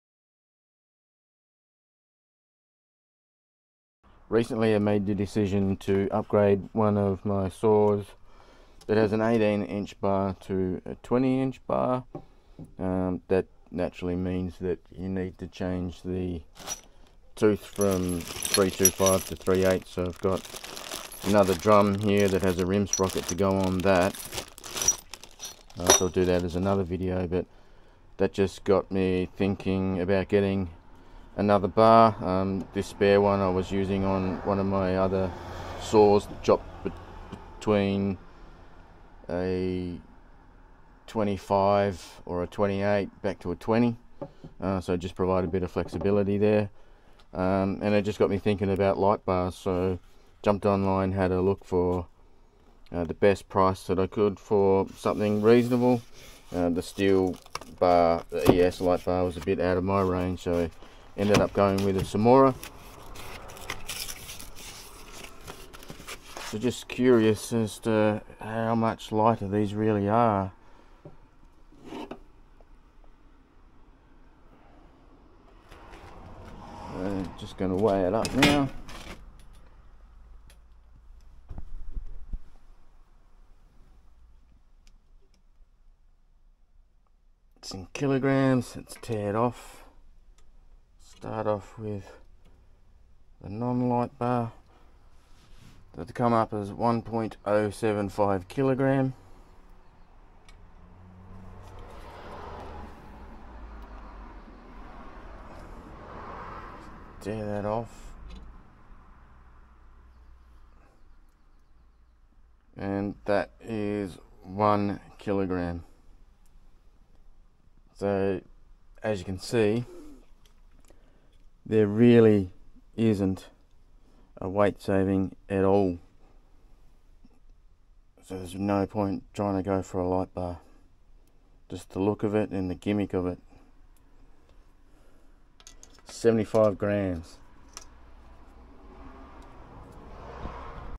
Lightbars, are they worth it!
Comparison of a STIHL Rollomatic E Chainsaw Guide Bar and a Tsumura Lightbar